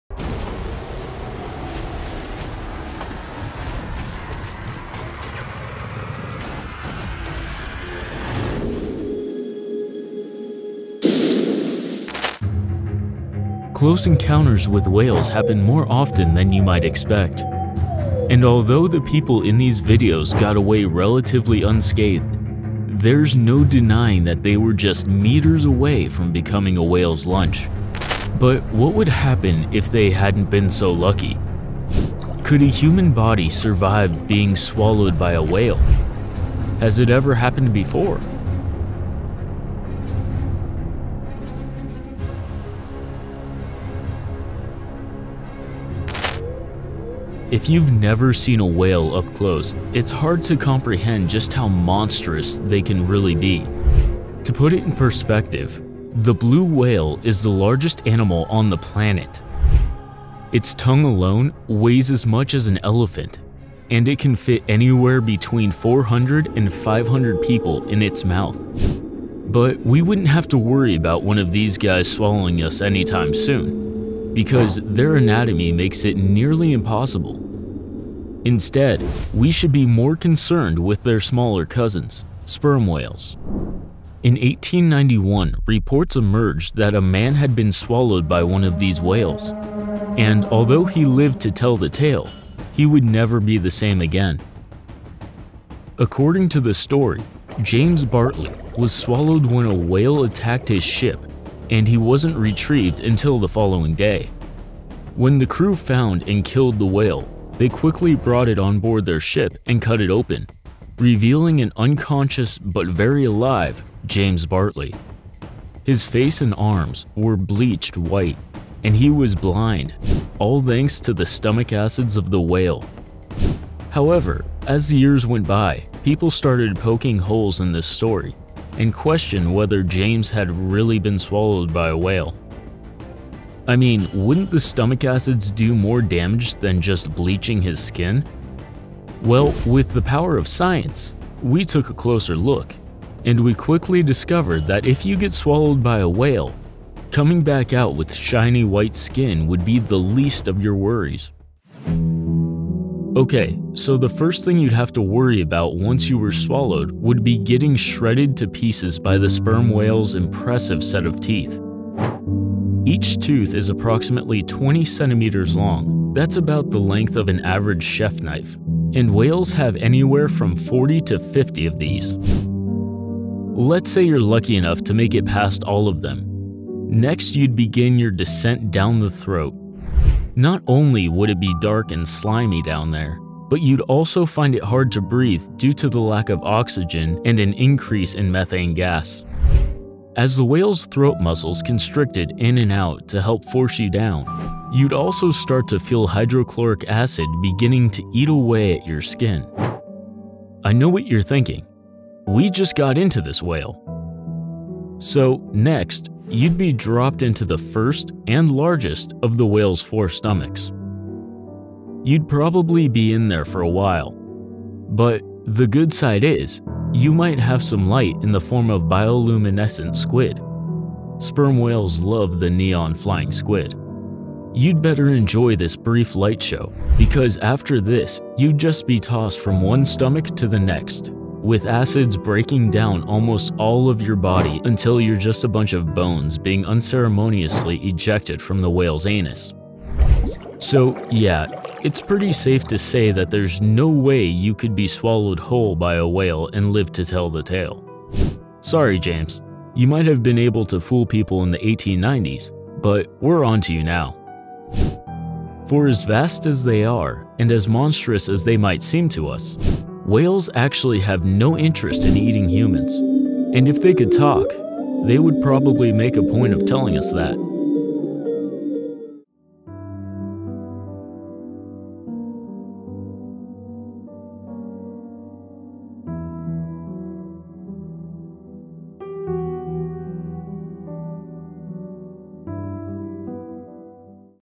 What If You Were Swallowed by a Whale?
Close Encounters  with whales happen more often than you might expect. And although the people in these videos got away relatively unscathed, there’s no denying that they were just meters away from becoming a whale’s lunch.
But what would happen if they hadn’t been so lucky? Could a human body survive being swallowed by a whale? Has it ever happened before?
If you’ve never seen a whale up close, it’s hard to comprehend just how monstrous they can really be. To put it in perspective, the blue whale is the largest animal on the planet! Its tongue alone weighs as much as an elephant, and it can fit anywhere between 400 and 500 people in its mouth.
But we wouldn’t have to worry about one of these guys swallowing us anytime soon, because their anatomy makes it nearly impossible. Instead, we should be more concerned with their smaller cousins, sperm whales.
In 1891, reports emerged that a man had been swallowed by one of these whales, and although he lived to tell the tale, he would never be the same again…